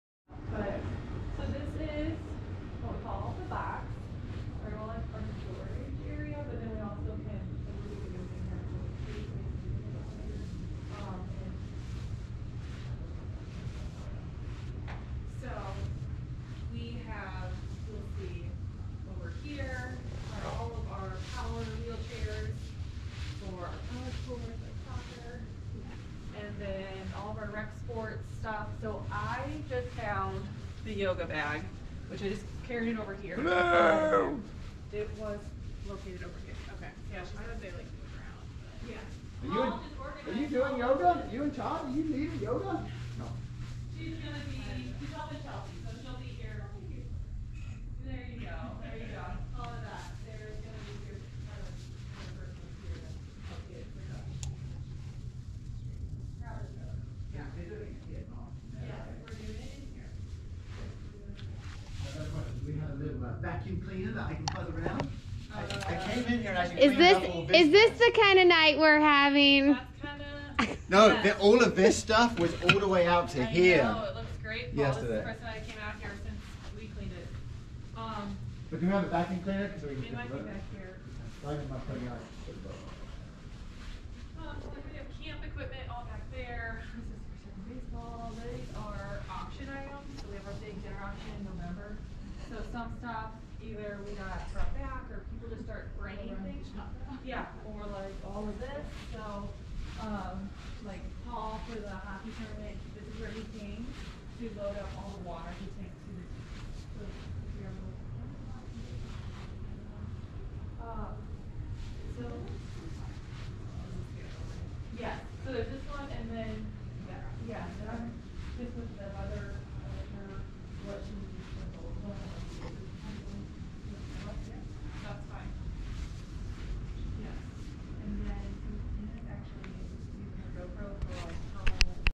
The Box Walkthrough, DASA's Storage Facility
Tour of The Box, DASA's Storage Facility and some of the adaptive equipment!
Tik-Tok: @dasa_adaptive_training
Zynn: @dasa_adaptive_training